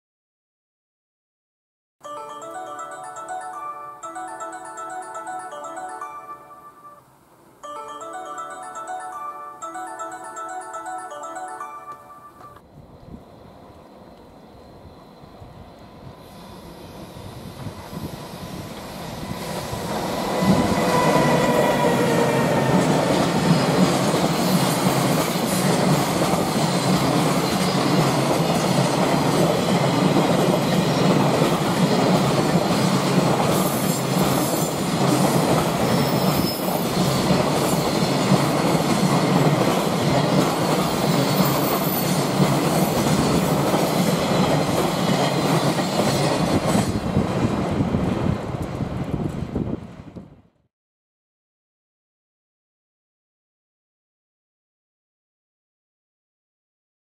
【本日の貨物列車】No.728 2022,7,15 第1062番列車 Today‘s freight train
本日の貨物列車は2022年2月11日に近江長岡~柏原駅間で撮影した第1062番列車です。